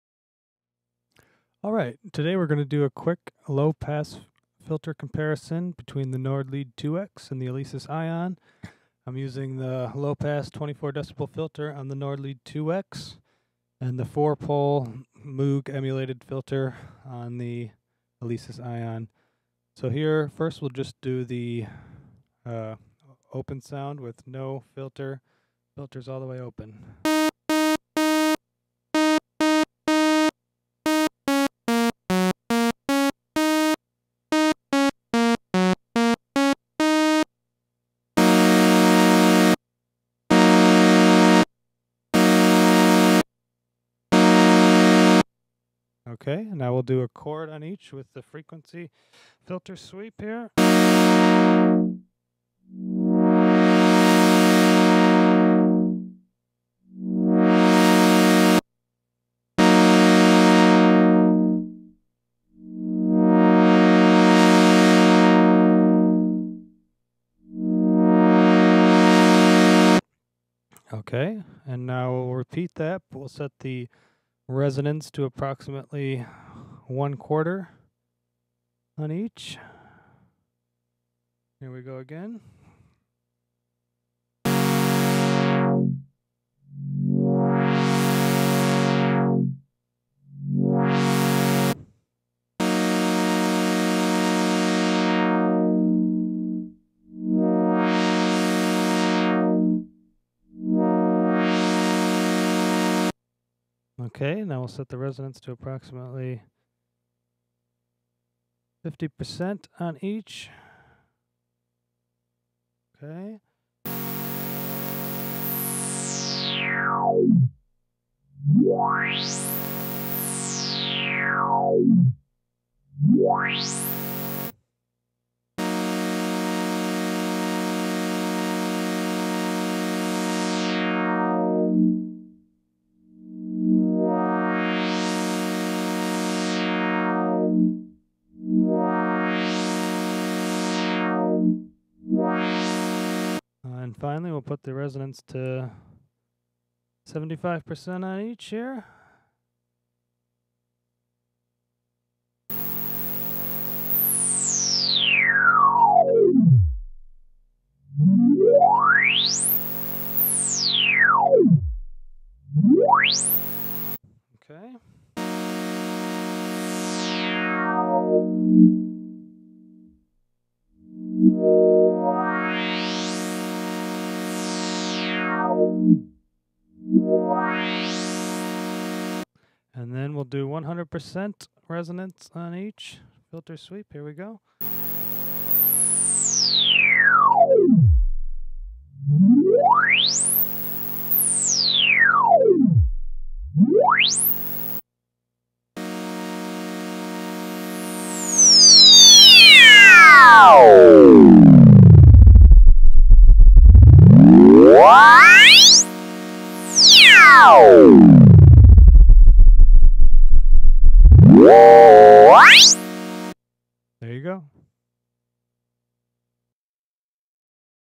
Alesis Ion & Nord Lead 2X - 24DB Low Pass Filter Comparison (HD)
A comparison between the 24DB Low Pass Filter of the Alesis Ion (analog modeling synth) and the Clavia Nord Lead 2X (virtual analog synthesizer).

Audio was recorded at 24bit 44.1K through a Steinberg MR816X interface into Logic and dithered to 16bit and combined with video taken with iPhone 5 in iMovie.

Note: The Ion has several low pass filters modeled, the one used for this comparison was the Moog style 4 Pole LP filter as it is the only one that provided a filter that went from 0-20K.